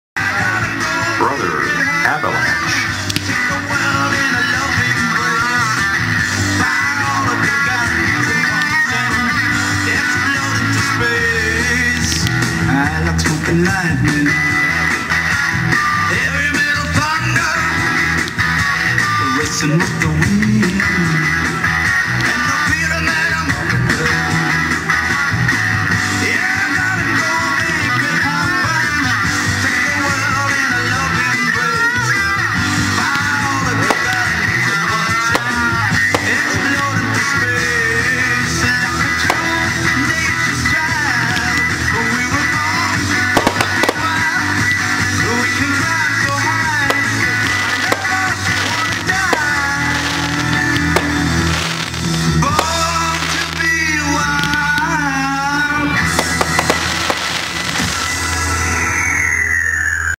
Avalanche
Silver Swirling Tail to Blue Bouquet Whistling Tail to Red and Green Bouquet. Fanstastic 5 shot finale!